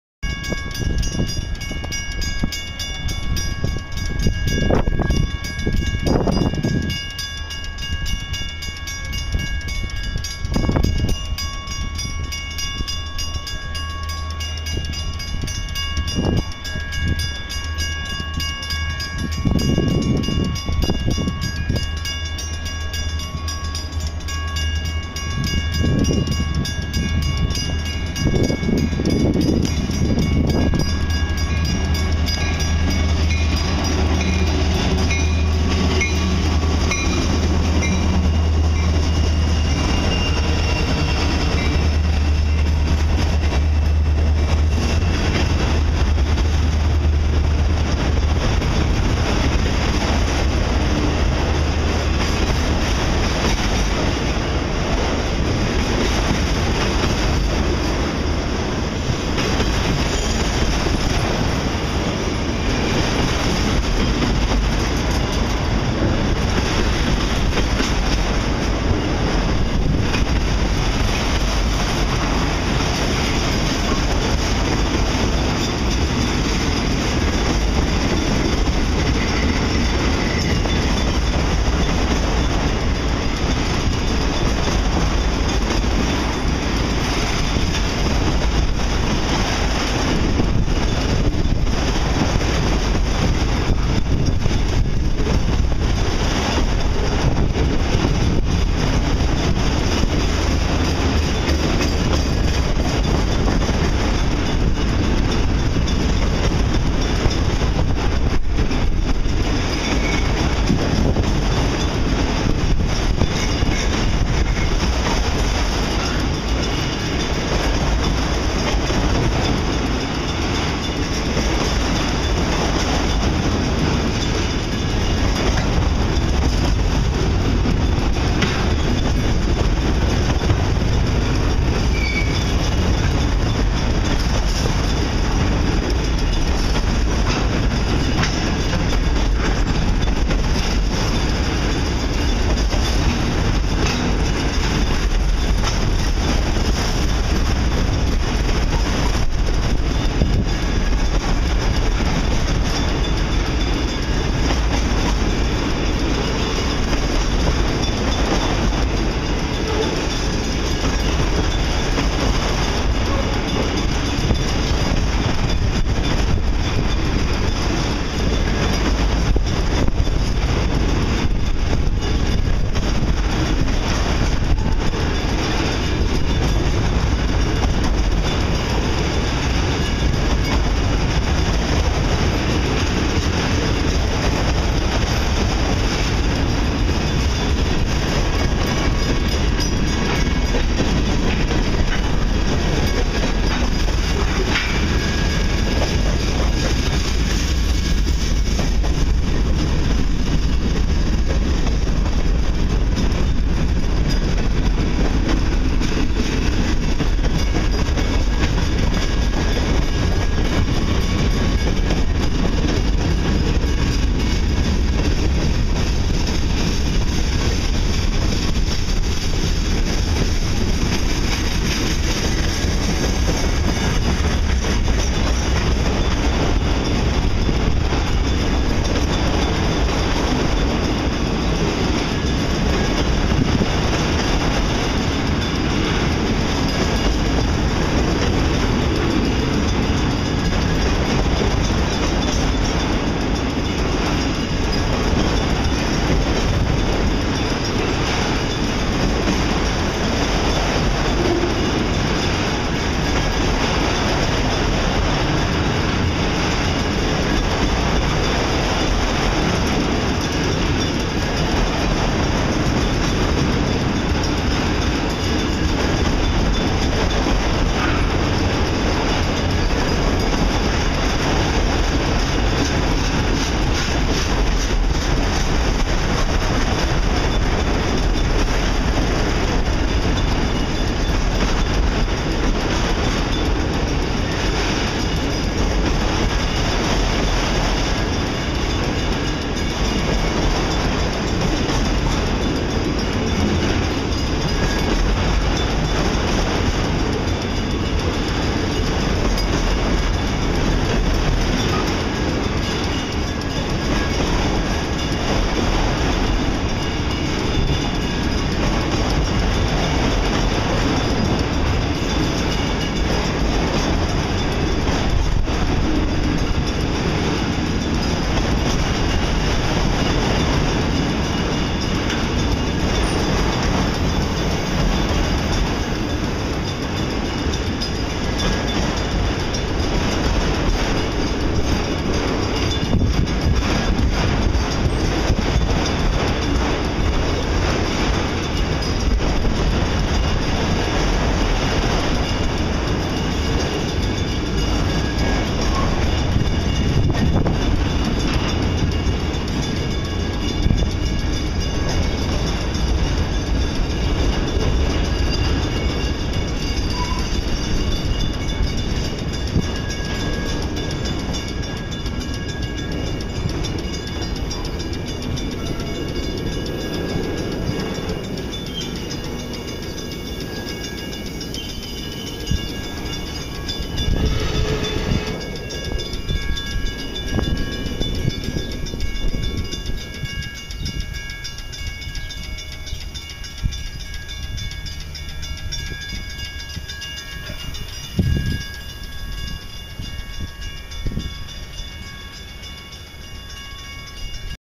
SOO 6024 at Northbrook, IL
SOO 6024 an EMD SD60 and HLCX 8033 an ex BN EMD SD40-2 loco leads an Eastbound freight at Northbrook, IL on the CP C&M sub. Recorded on 5/6/2011